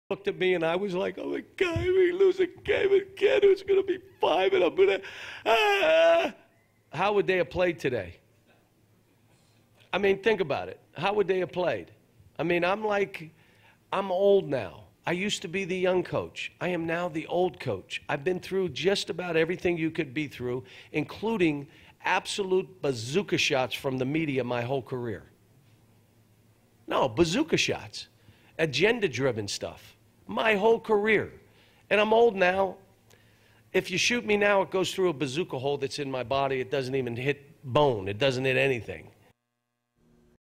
"If you shoot me now, it goes through a bazooka hole."
LEXINGTON, Ky (WTVQ) - With their backs against the wall, the Kentucky Wildcats Basketball team beat Alabama on Saturday, 81-71.
After the game, Coach Cal talked about how he's kept his composure for his team.
He also went into how he's taken "bazooka shots" from agenda-driven media during his time as a coach.
Check out the video for all his comments.